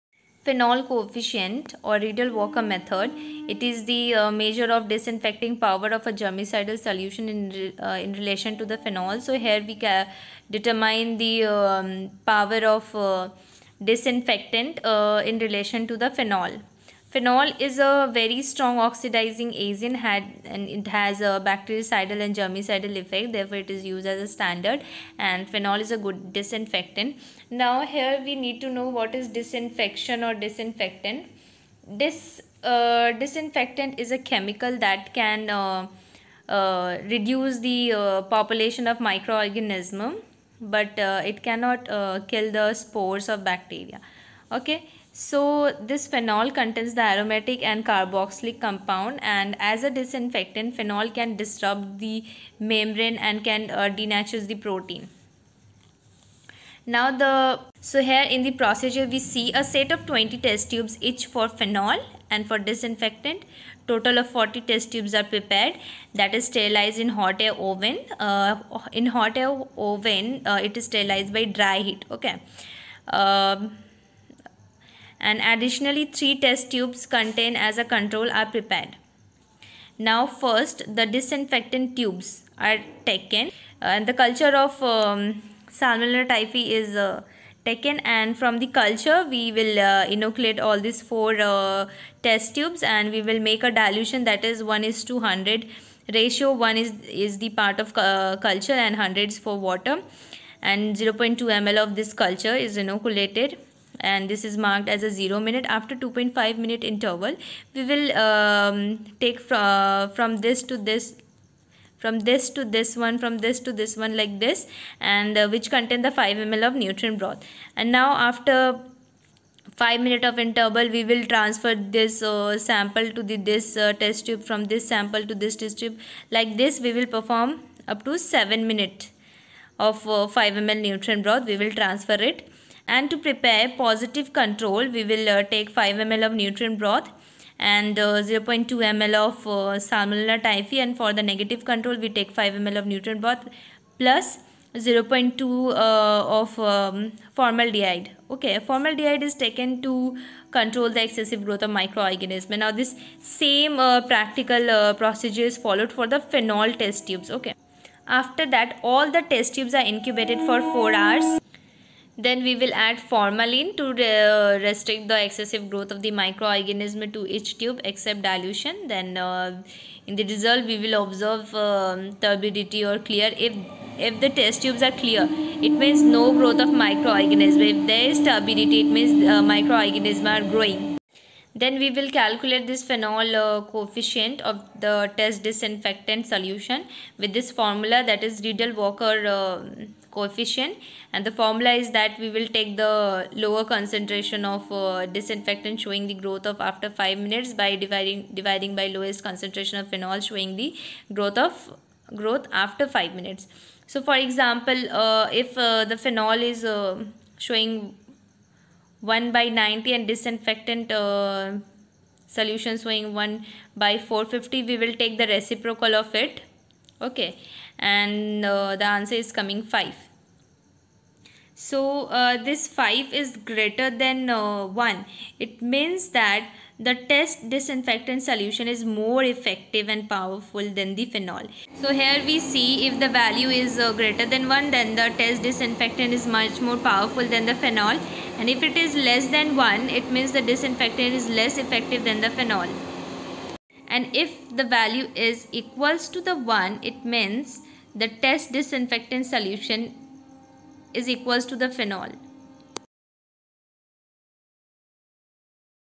phenol coefficient test | Rideal Walker coefficient test
It measures the power of disinfectant solution in relation to phenol .